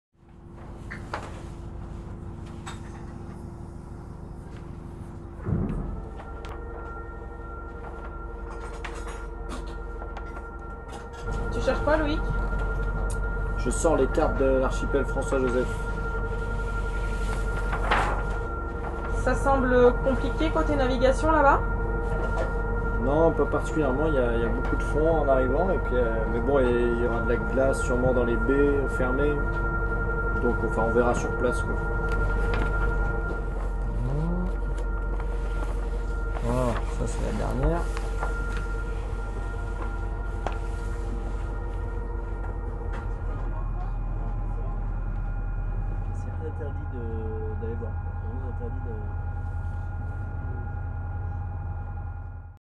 Le capitaine se prépare pour François Joseph - Expédition Tara Oceans Polar Circle - 4 août 2013
Le capitaine étudie les cartes pour naviguer dans l'archipel de François Joseph.